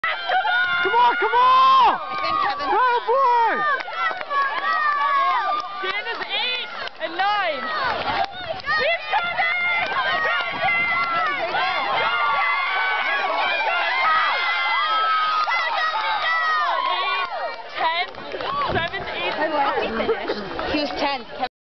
Leger lancers sr boys Ropssaa cross country champions
Top racers on the Cardinal Leger Lancers cross country team bringing home gold in 2008 Ropssaa Championship